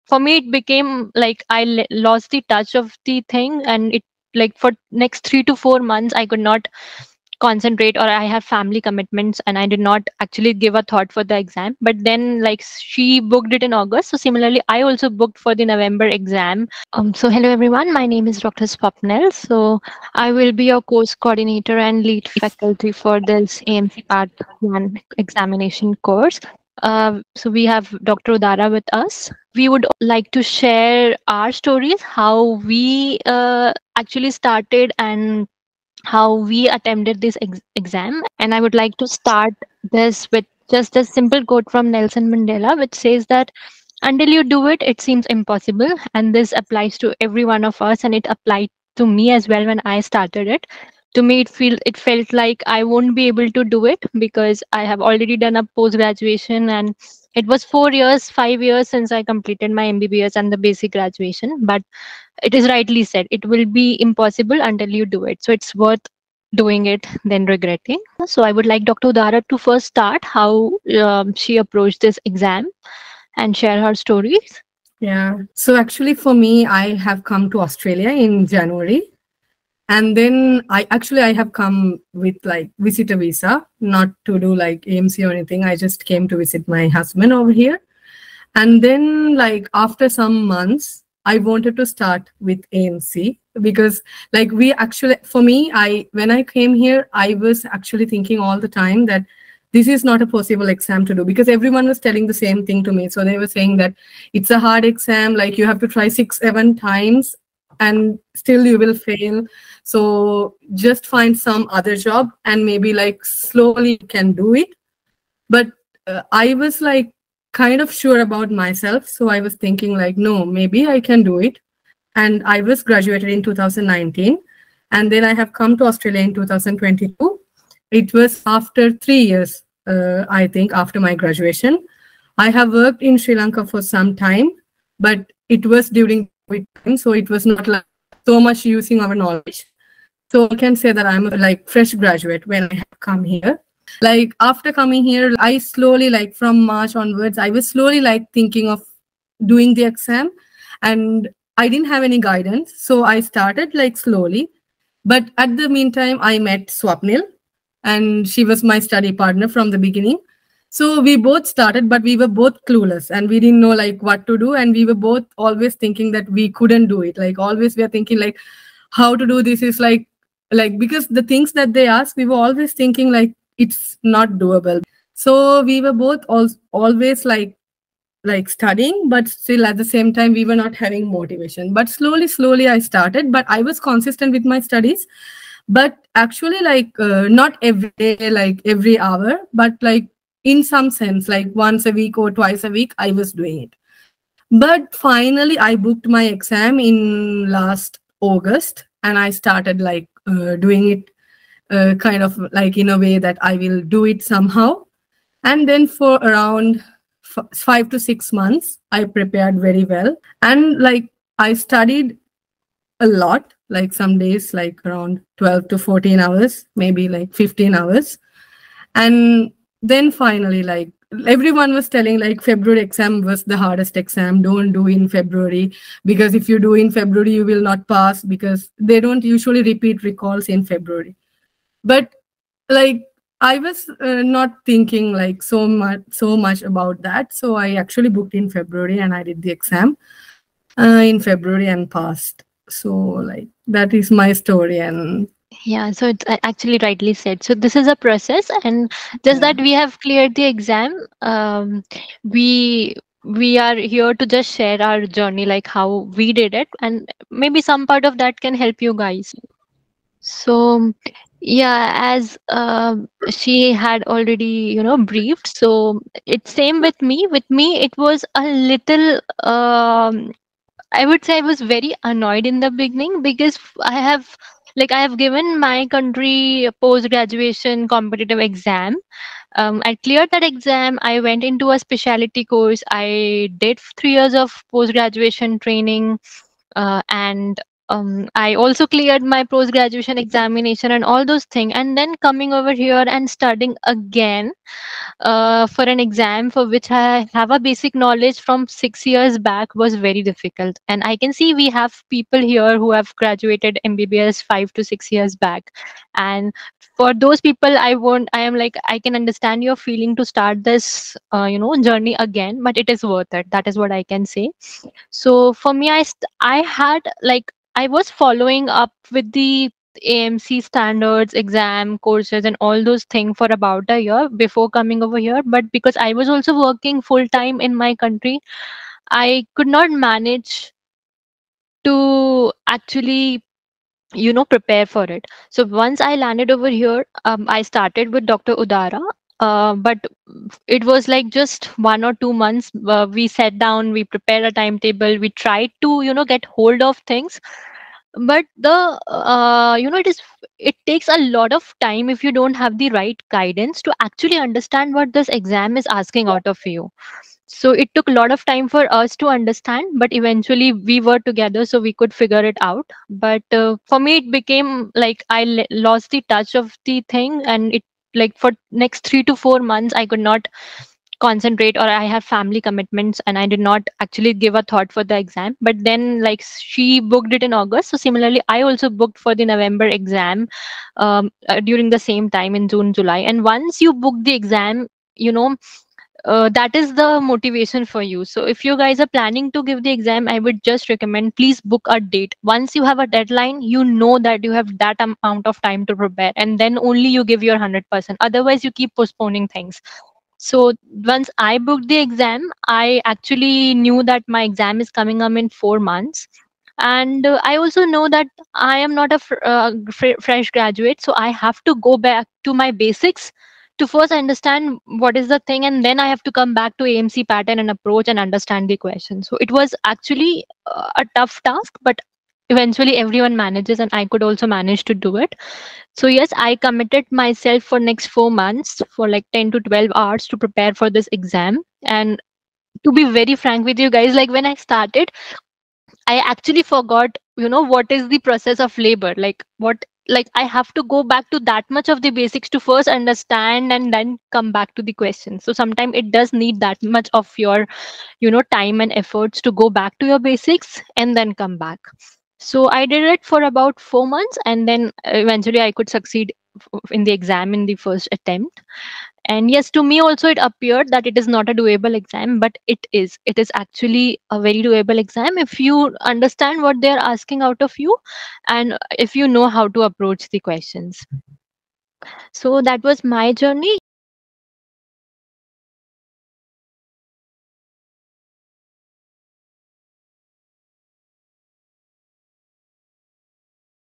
Becoming Registered Doctor in Australia | Successfully Crack AMC Exam | MedPrep Academically
Listen to the success stories of Dr Swapnil and Dr Udara who aced the Australian Medical Council Exam with flying colours. Working as a doctor in Australia is not impossible for you.
If you are an MBBS doctor, MS doctor or an MD doctor, you can also build your dream career in Australia by clearing the AMC exam.
Watch to learn more about the challenges, process, and preparation of the AMC exam.
If you dream of working as a doctor in Australia, you have come to the right place. From the best AMC MCQ preparation to a smooth migration assistance, we have got you covered.

Chapters:
00:00 Introduction | Sharing AMC journeys
01:43 Preparing for AMC exam
03:44 Booking the AMC exam
04:27 Revisiting medical basics
06:51 Committing to AMC prep